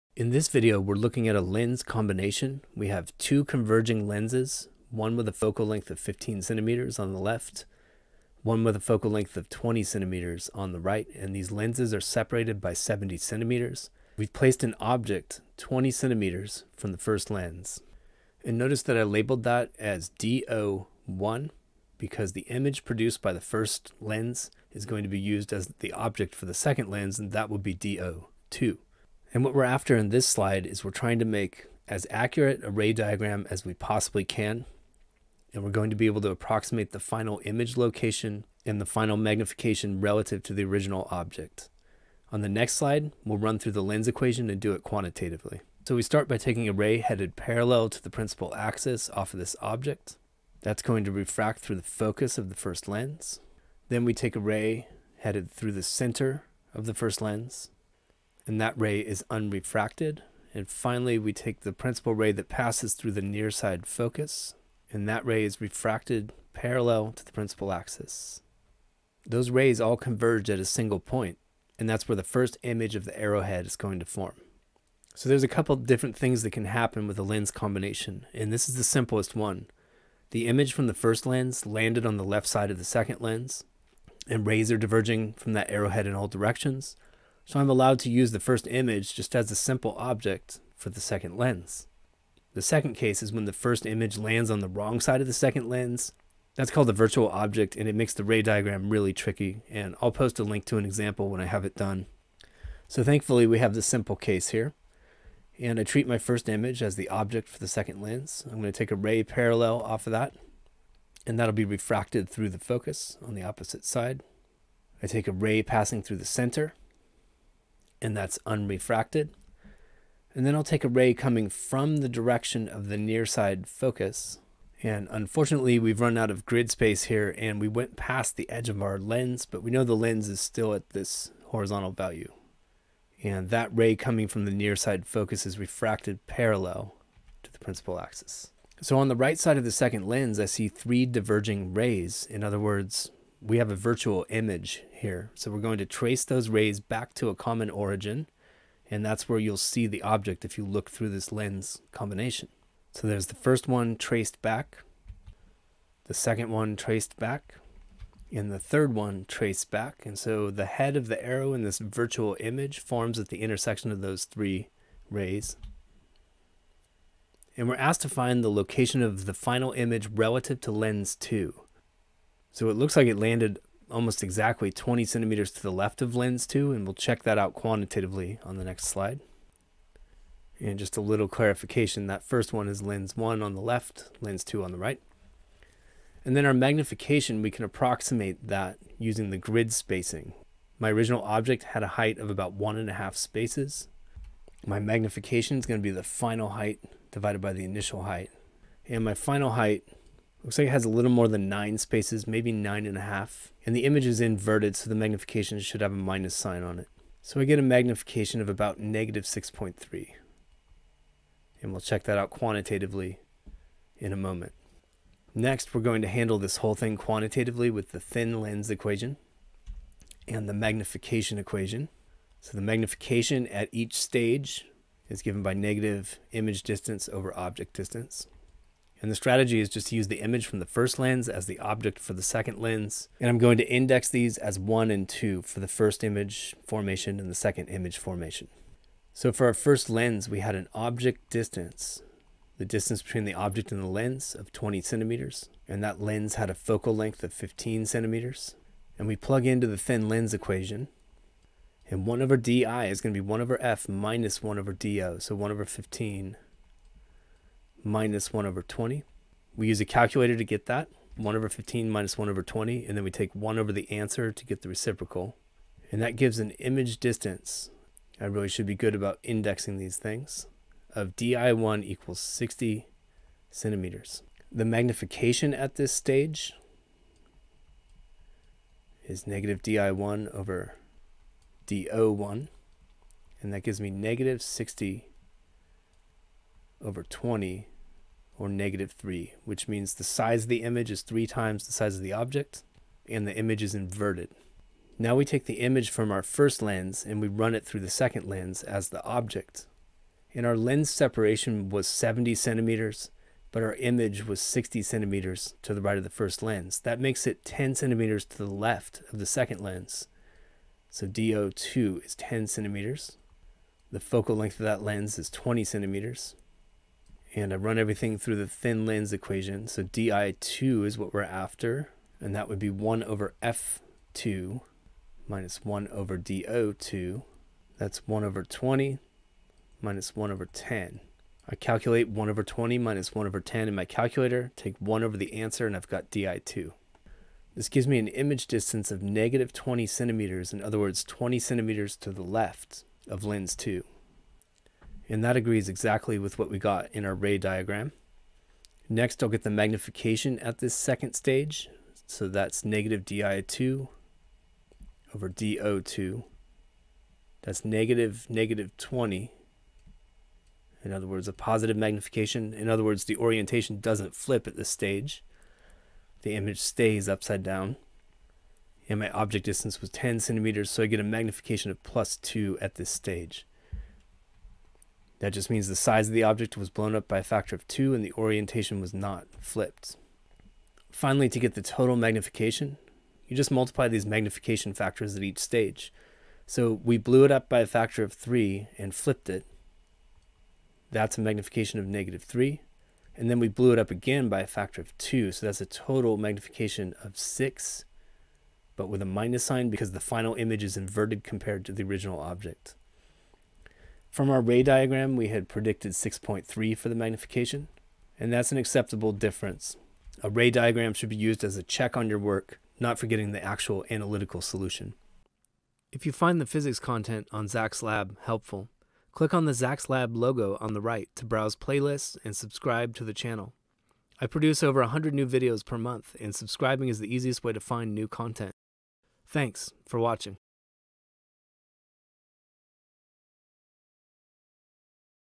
Image formed by two lenses, apply the lens equation twice. Image distance and magnification.
In this image formed by two lenses problem, we apply the lens equation twice and find the final image distance and magnification.

We start with the ray diagram for two converging lenses.  We take all three principal rays off the object and pass them through the first lens to form an inverted real image on the left side of the second lens.

This is the easy case of two different possibilities:  when the image lands on the left side of the second lens, we use the first image as the second object.  The harder case is when the first image lands on the opposite side of the second lens.  That's a virtual object, and we'll do an example of that in another video.

We take all three principal rays off the first image and run them through the second lens.  We find that the principal rays diverge, which means we are dealing with a virtual image.  We trace the principal rays back to a common origin, and this is where the inverted virtual image of the arrow's head will form.  We read off the grid to approximate the image distance using a ray diagram, and we count grid spaces to approximate the magnification using a ray diagram.

To analytically find the image formed by two lenses, apply the lens equation twice.  We index the first object with a 1 and find the first image distance using the lens equation.  We calculate the second object distance based on the lens separation.  Then we apply the thin lens equation a second time to find the image distance relative to the second lens.  This agrees exactly with the approximation from our ray diagram.

After repeated use of the magnification equation we find the magnification for two lenses by multiplying the magnifications at each step.  We obtain a magnification slightly different from the ray diagram, but close enough!